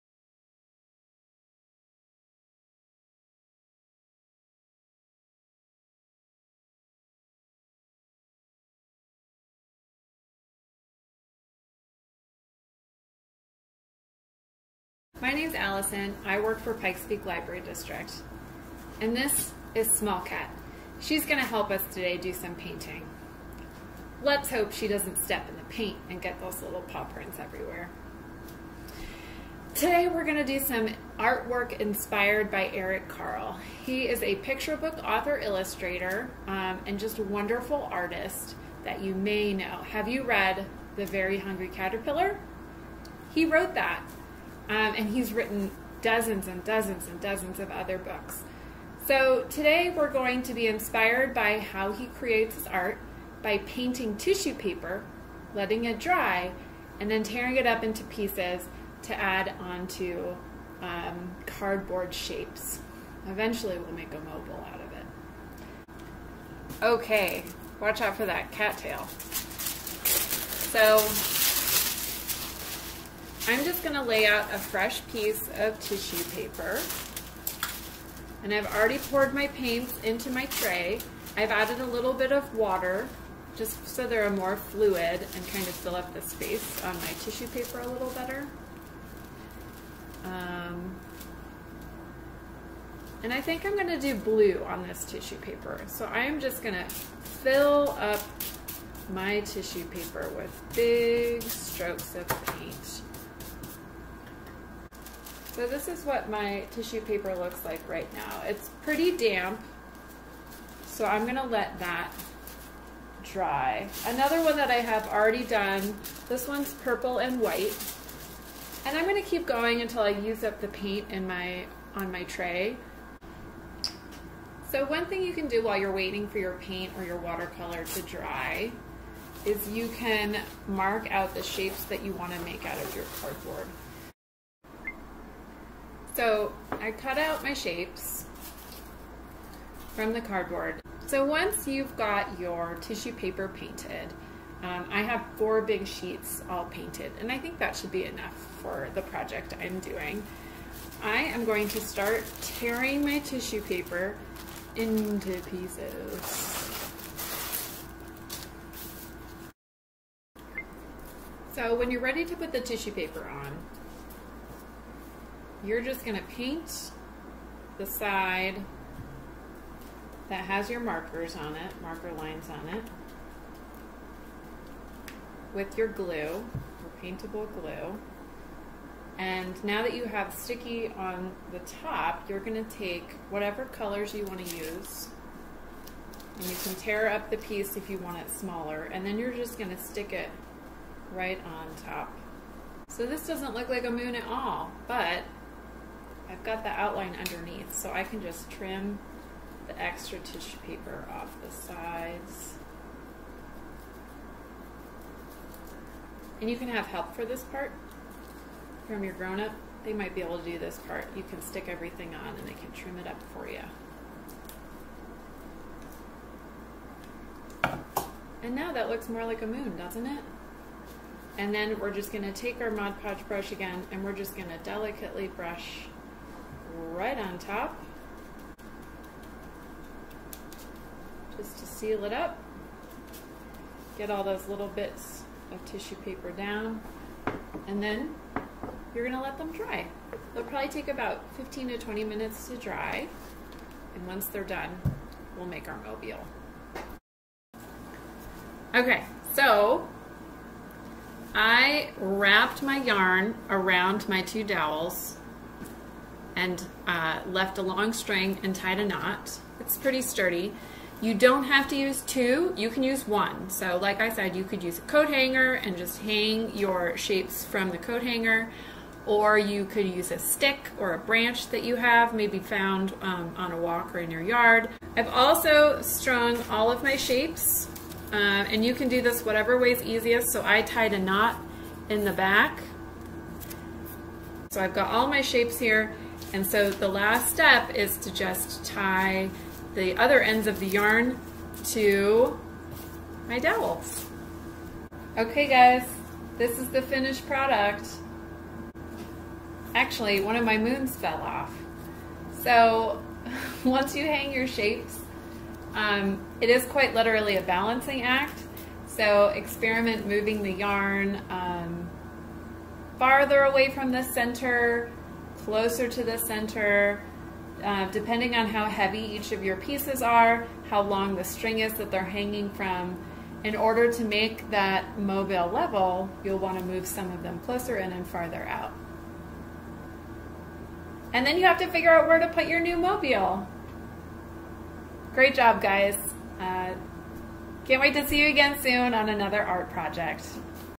PPLD KidsMAKE: Eric Carle Inspired Painted Collage Mobile
Taking inspiration from the popular children's book author and illustrator Eric Carle, Allison will show us how to paint tissue paper and then tear it up into pieces to collage onto cardboard shapes. Finally, we'll hang our shapes from a dowel to make a mobile. For supplies and directions for this project,